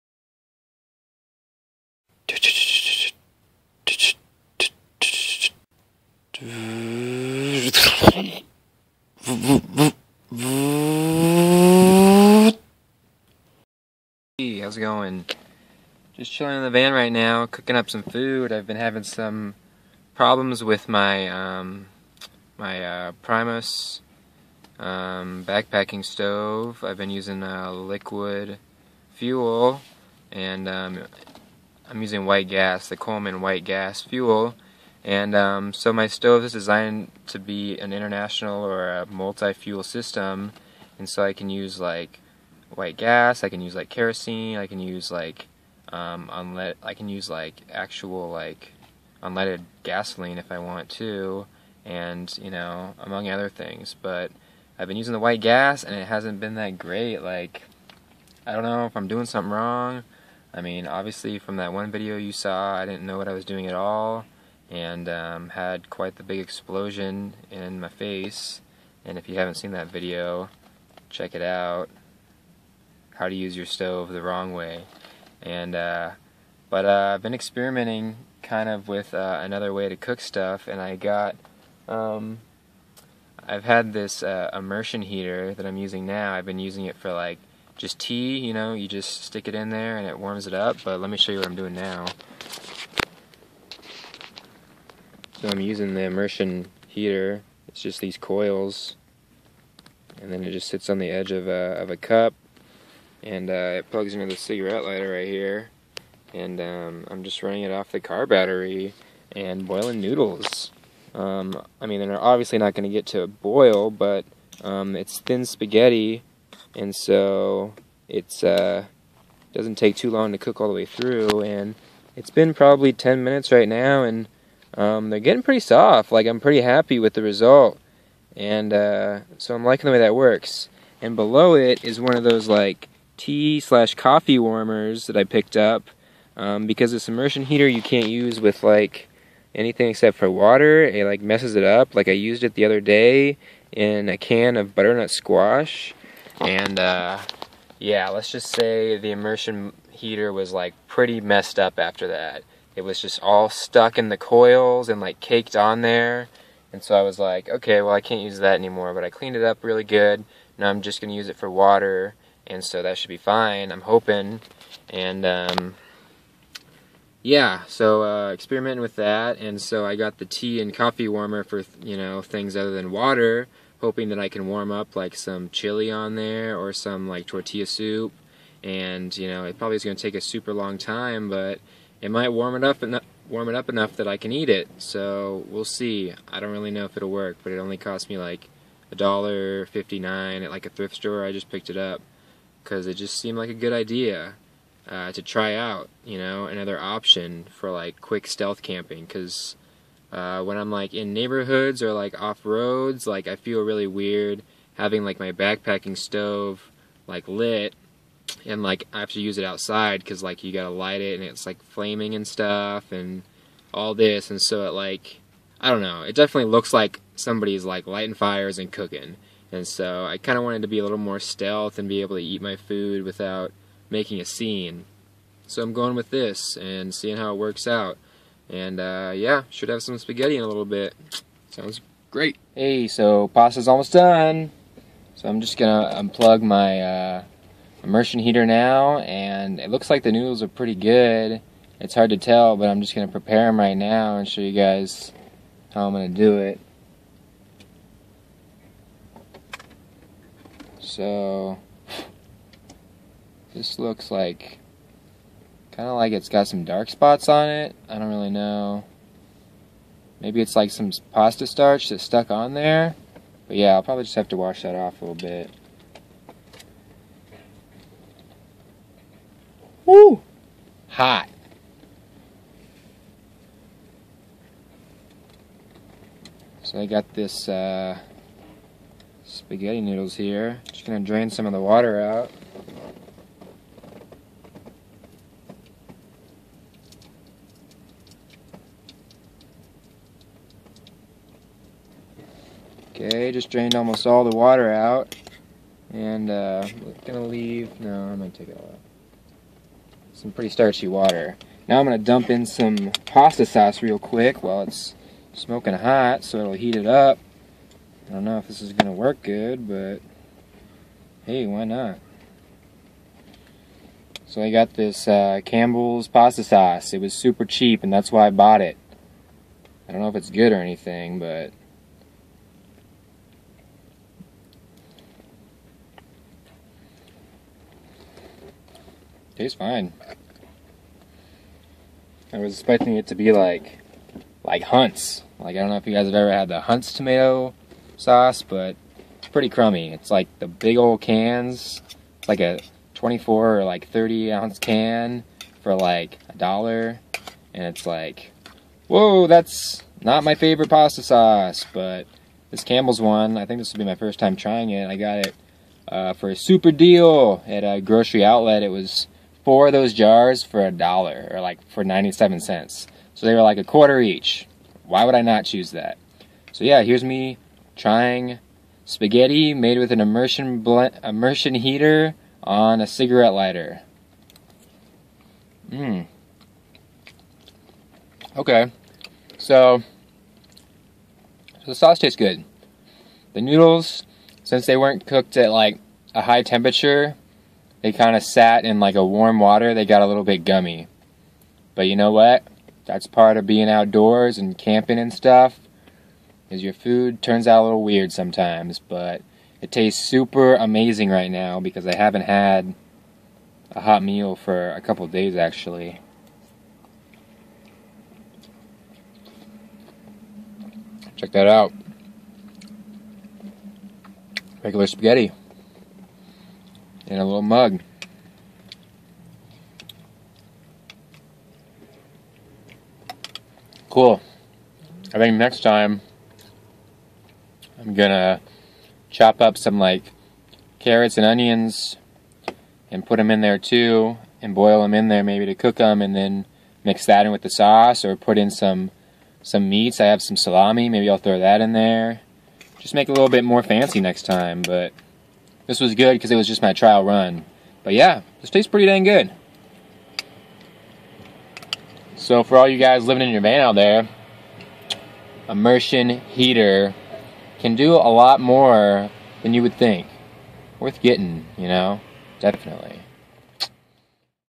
Culinary experiences in a Van: Spaghetti
Trying out alternative cooking methods living in a van. Cooking up a little spaghetti in the most unlikely of ways.